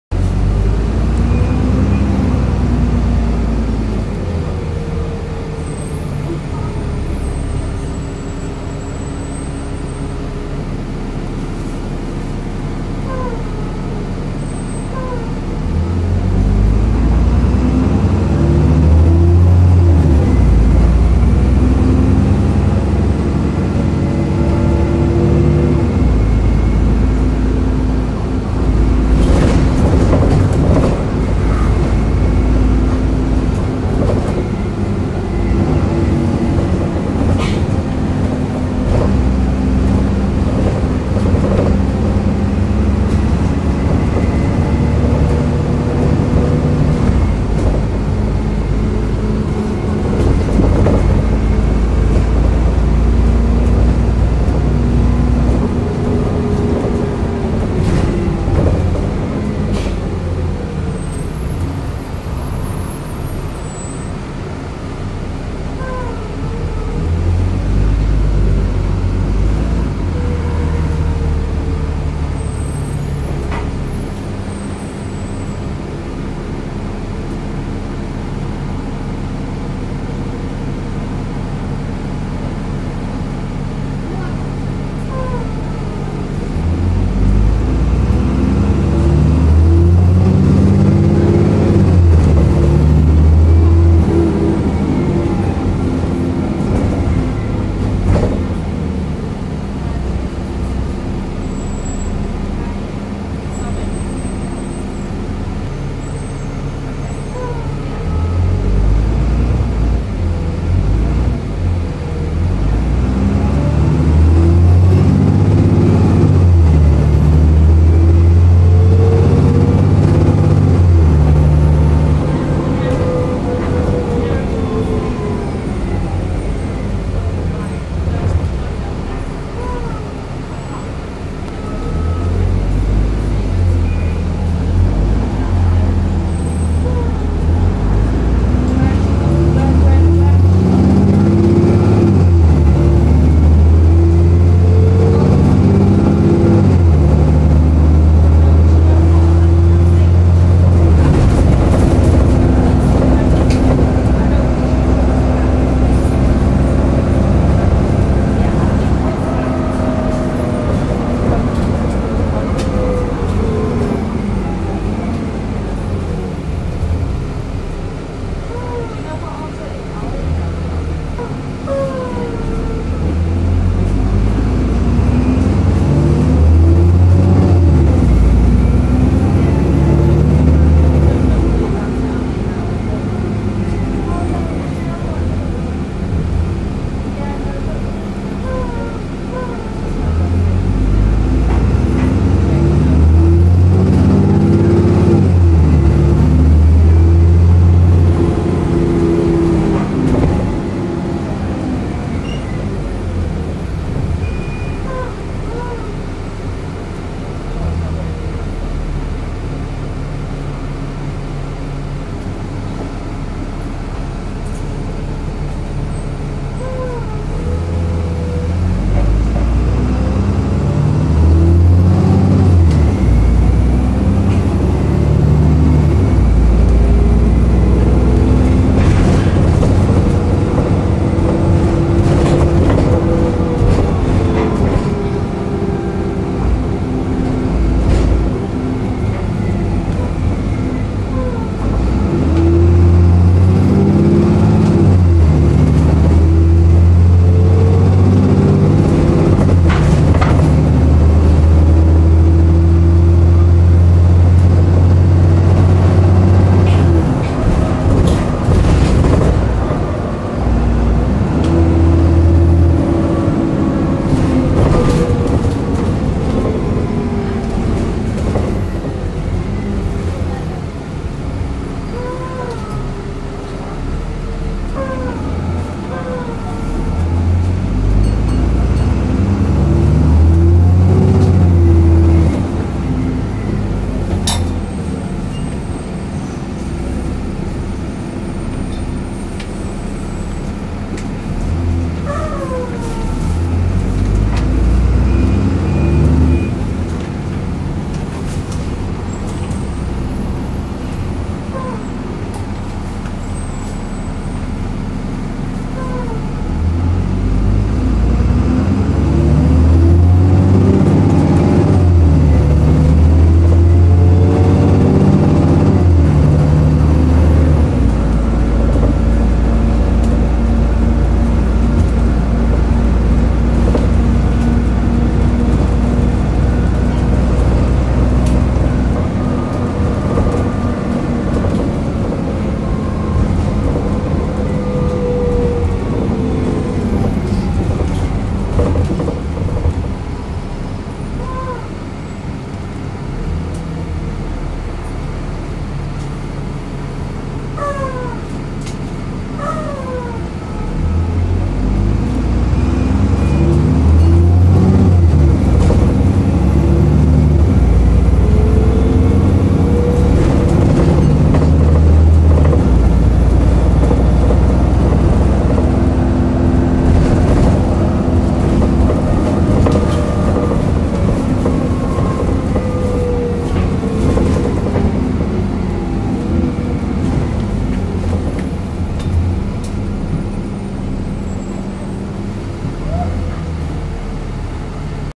Service: 143 | Stagecoach Manchester Alexander Dennis Enviro 400 19165 (MX07 HNG) some kickdowns
Dennis Trident II/Alexander Dennis Enviro 400
Service 143: West Didsbury - Manchester Piccadilly (Magic Bus)
Date filmed: 19th July 2016

This bus also makes loud groaning noises while stationary.

Taken during the hottest day of the year so far with temperatures peaking at 32C.